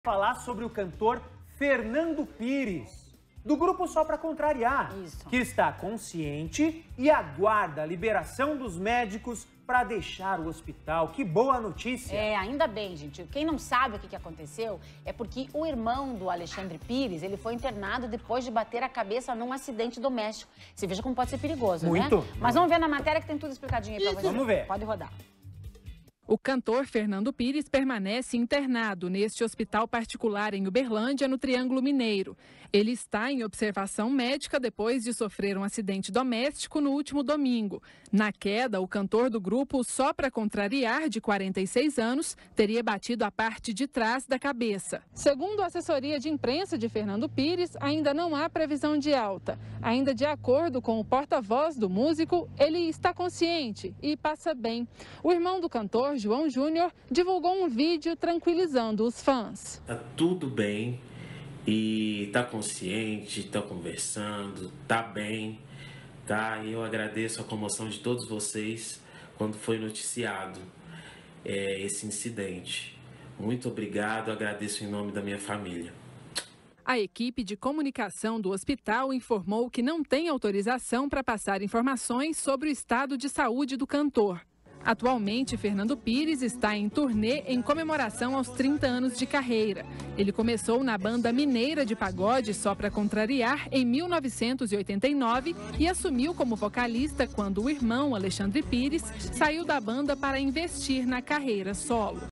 Fernando Pires bate e cabeça em acidente e está internado
O Cantor Fernando Pires, do grupo ‘Só pra Contrariar’, está consciente e aguarda liberação dos médicos para deixar o hospital, em Minas Gerais. Veja!